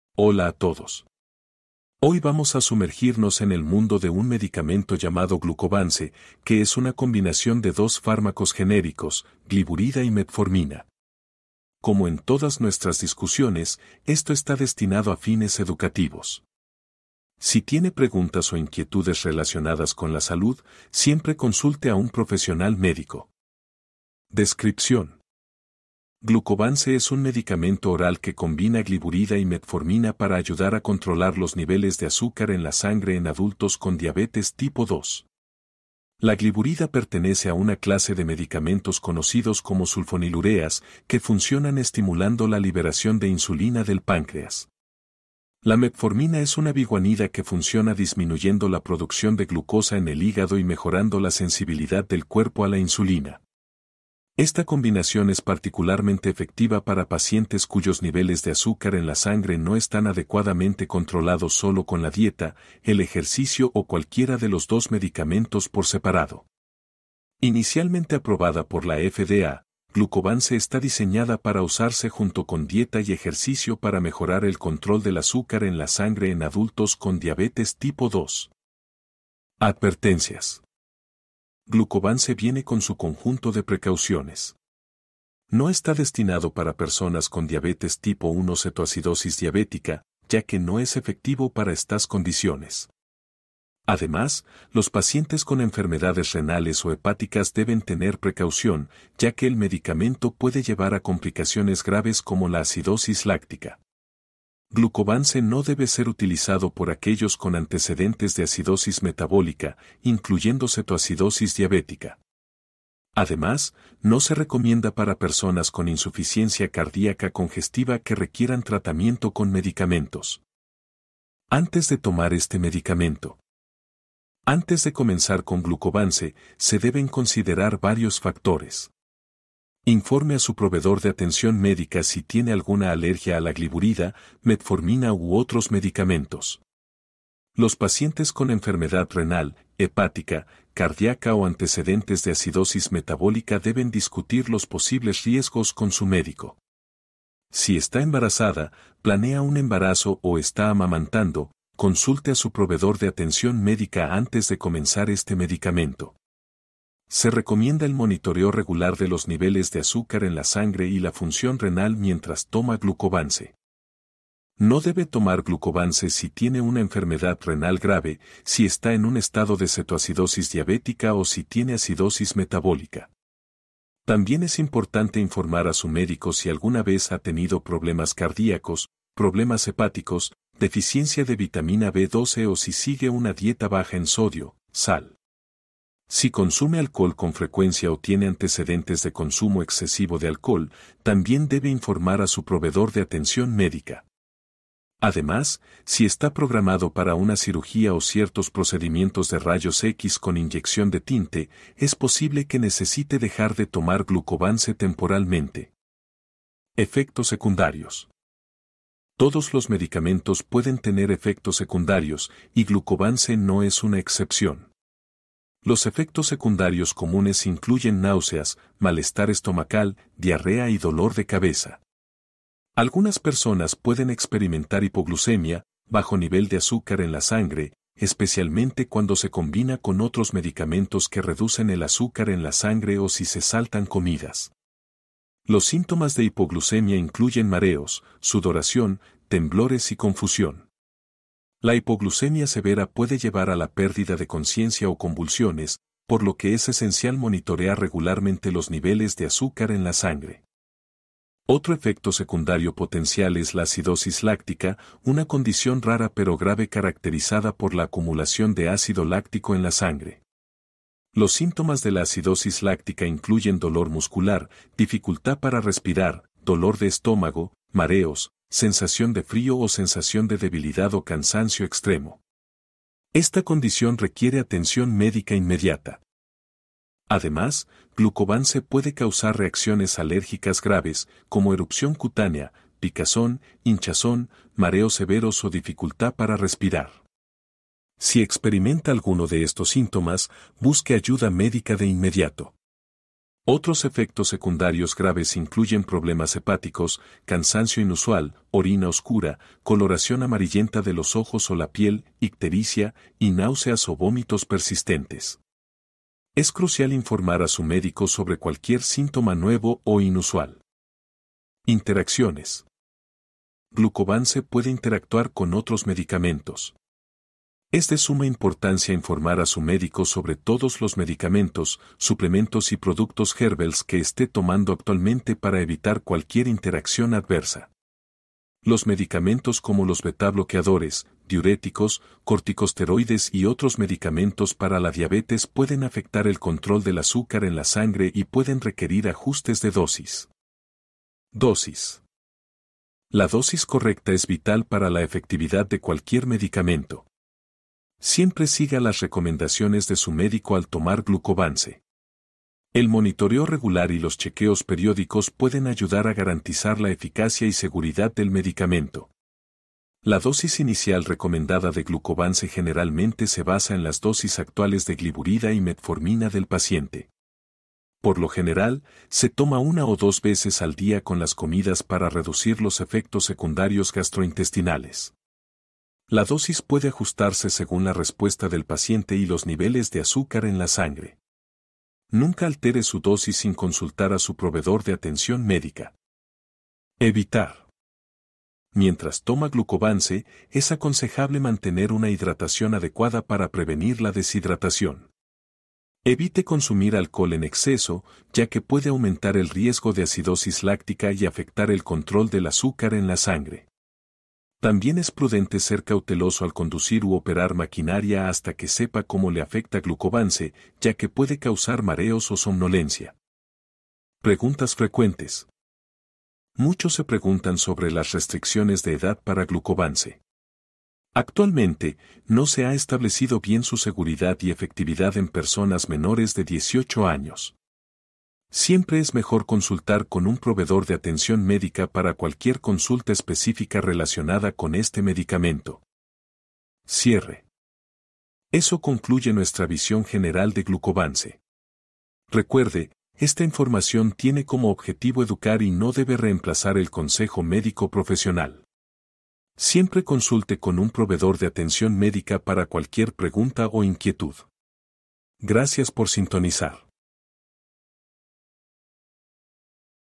Glucovance (gliburida y metformina) - Drug Rx Información (Spanish/Español)
Información general sobre Glucovance (gliburida y metformina). Contenido creado utilizando inteligencia artificial con el objetivo de conectar el acceso entre comunidades. Por favor, señale cualquier sugerencia o error en la sección de comentarios. ¡Gracias!